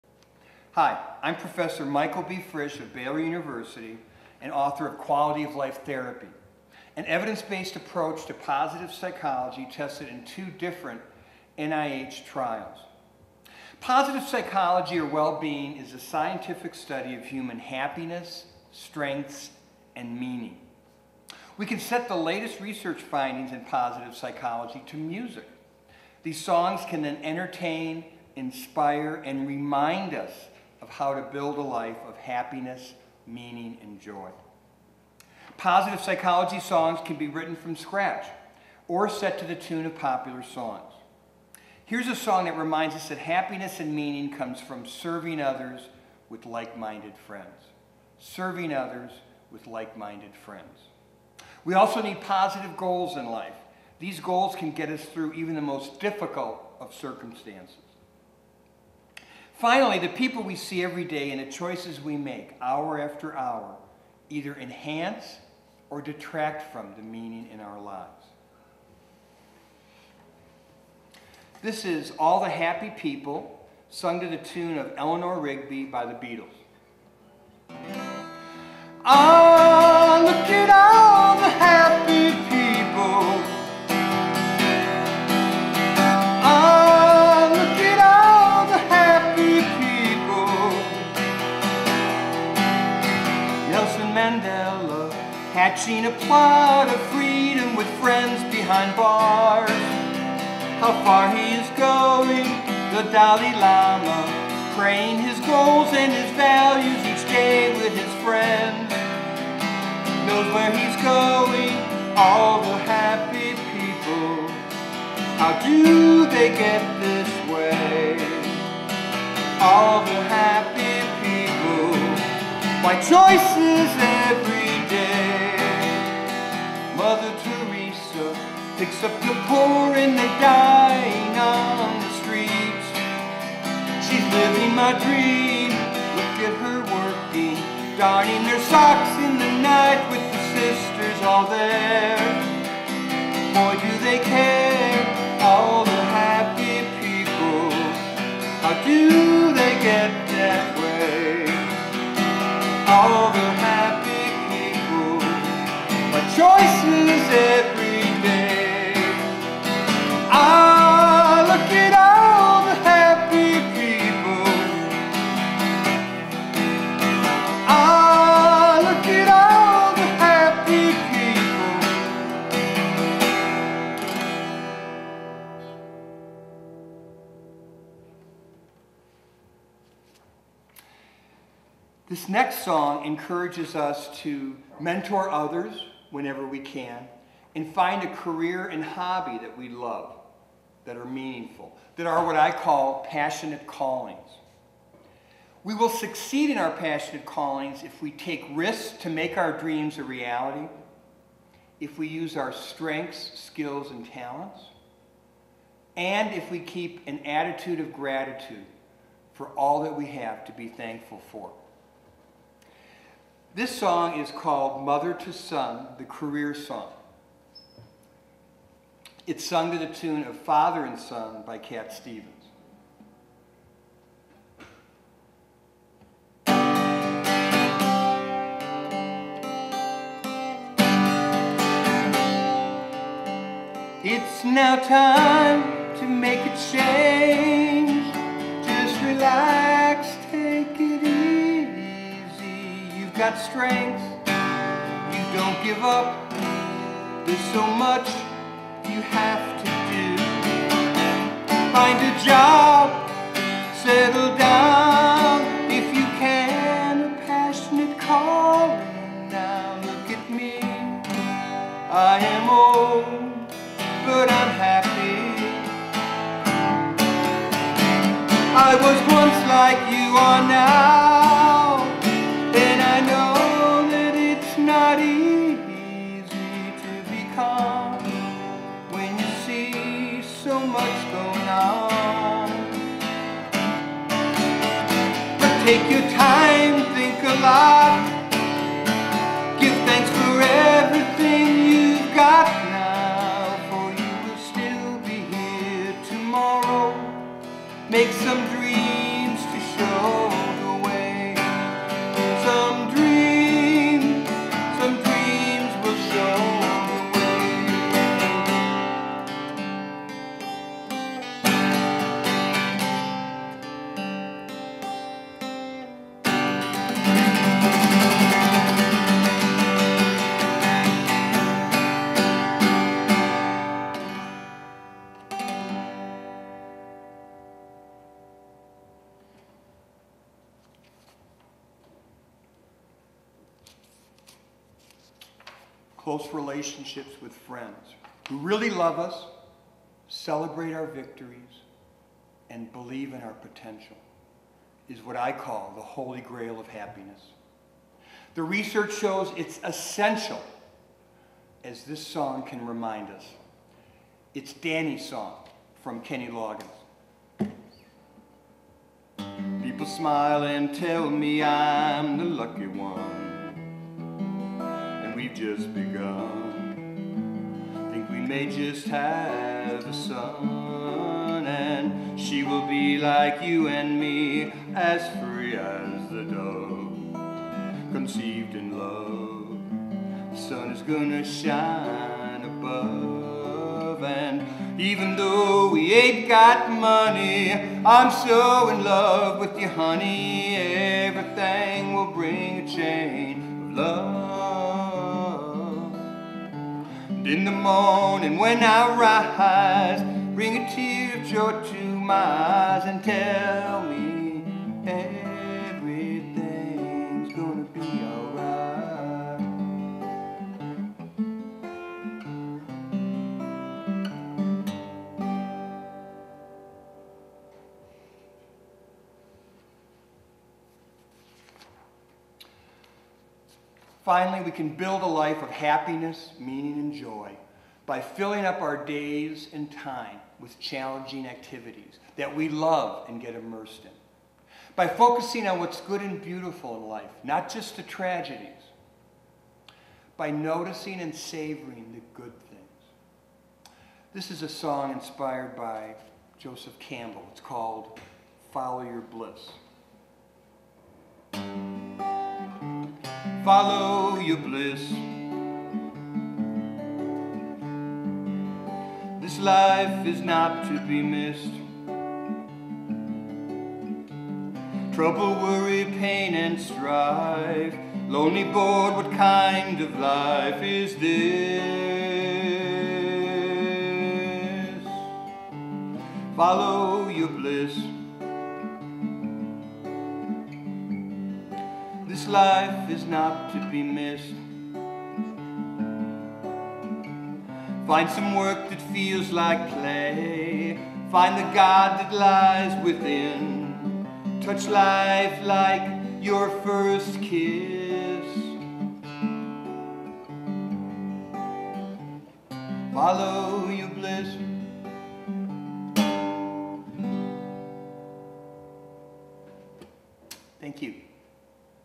How to be Happy Songs from Well-Being and Positive Psychology by Professor Michael B. Frisch
Positive Psychology/well-being is the scientific study of human happiness, meaning and strengths.

Professor Michael B. Frisch of Baylor University, author of Quality of Life Therapy, sets the latest research findings to music.  Positive psychology  songs can be made up by anyone.  They can  entertain, inspire, and remind us how to build a life of happiness, meaning, joy and success.  These songs are dedicated to Marty Seligman, Chris Peterson,  and Ed Diener, leaders of the positive psychology movement.